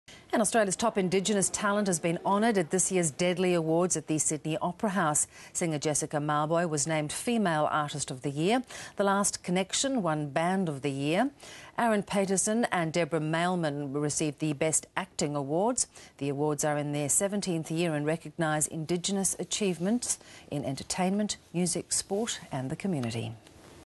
Deadlys take place in Sydney
Australia's Indigenous talents have been rewarded at the Deadly Awards at the Sydney Opera House.